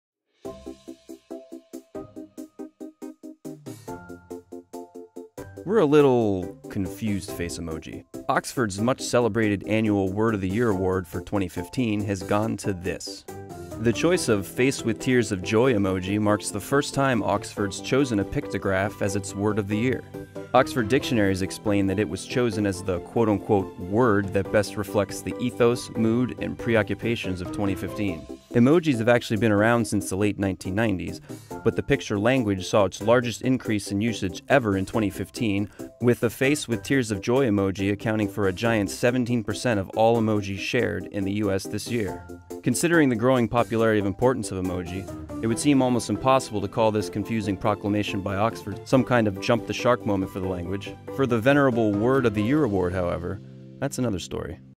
Trending Tech: The Word of the Year is an Emoji
A picture wins the Oxford word of the year.